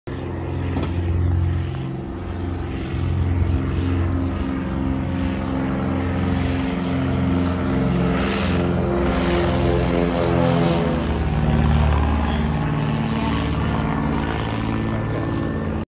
Britten-Norman BN2A-26 Islander Takeoff From Block Island State Airport
Operated by New England Airlines